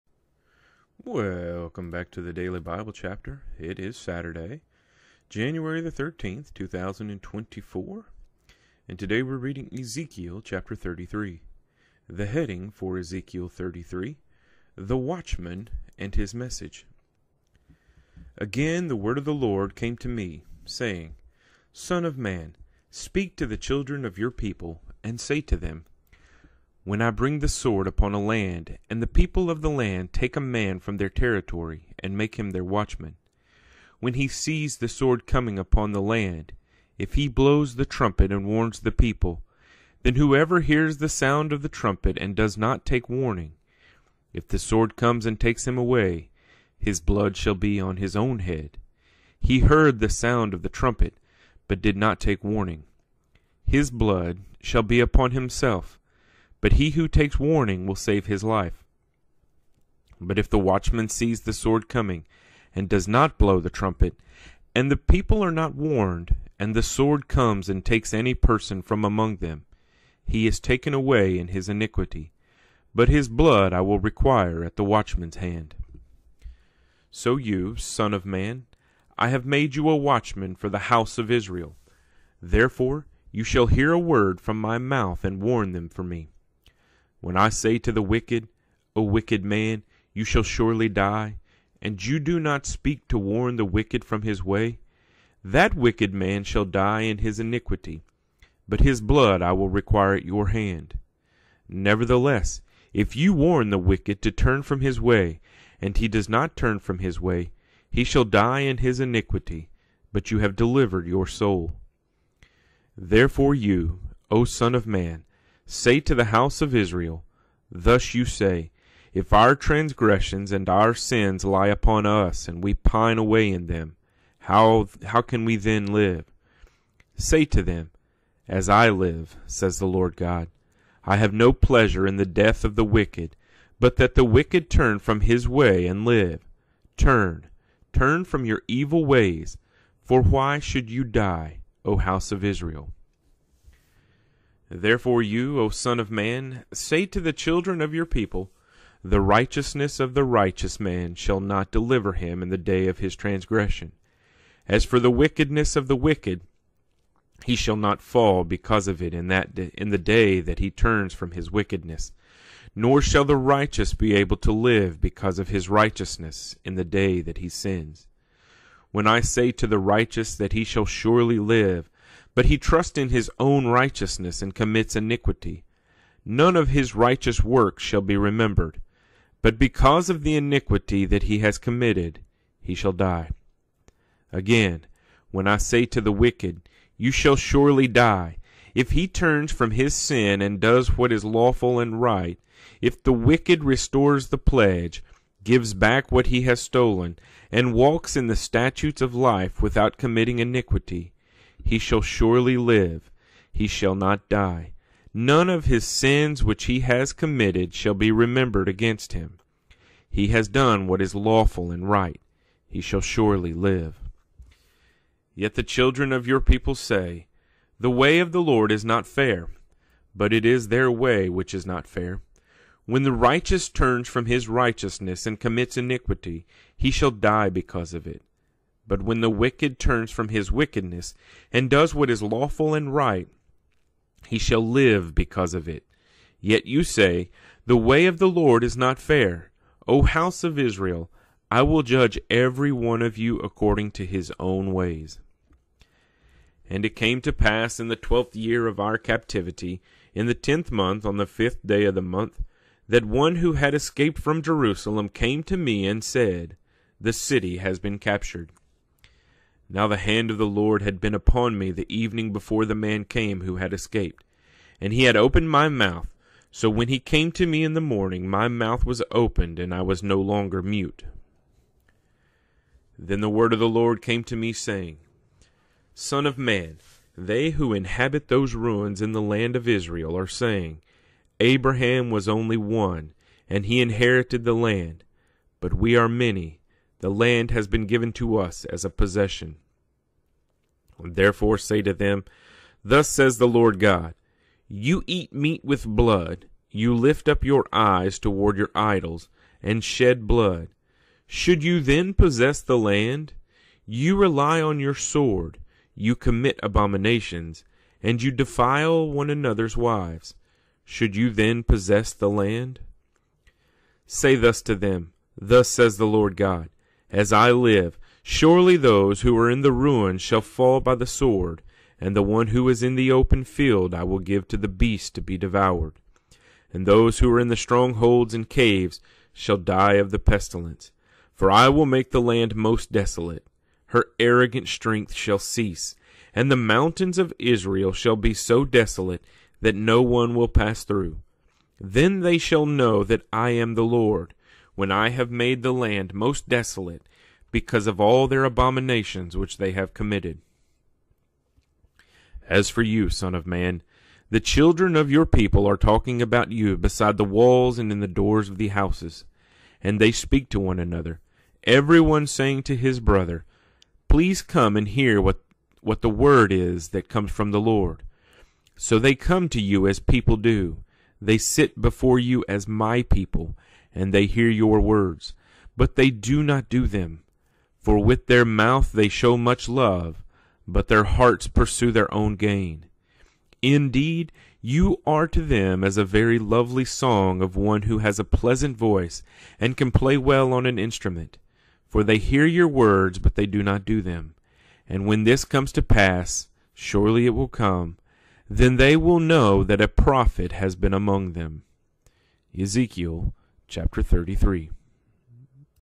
Ezekiel 33 - The Watchman and His Message
Today we are reading Ezekiel chapter 33 from the New King James Version (NKJV)